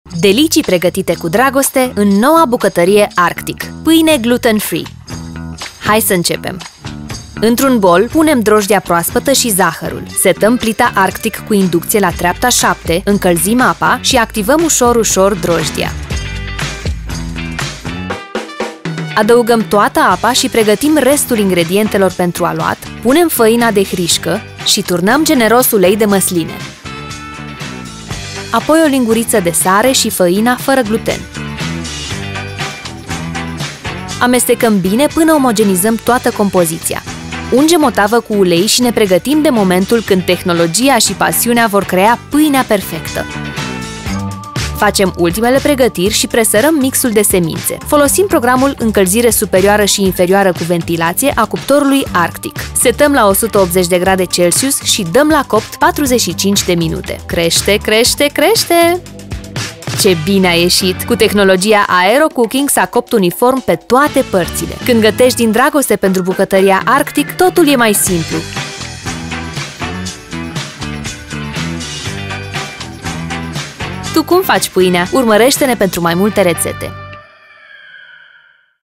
Reteta gluten-free: Paine - Din dragoste pentru noua bucatarie Arctic
Suntem siguri ca painea gluten-free va fi vedeta meselor tale. 😍 Hai sa iti aratam cum sa o prepari! 👨‍🍳 Cand gatesti din dragoste pentru electrocasnicele Arctic, totul este mai delicios!
Urmareste intreaga reteta!

Ingrediente:

Maia
✅  25g drojdie proaspata
✅  1 lingura zahar
✅  50ml apa calduta

✅  50g faina hrisca
✅  40ml ulei de masline
✅  400ml apa calduta
✅  400g faina gluten-free
✅  1 lingura sare
✅  20g mix de seminte

- Temperatura cuptor: 170-180 Grade
- Functie cuptor: incalzire superioara si inferioara cu ventilatie
- Timp de coacere: 40-50 minute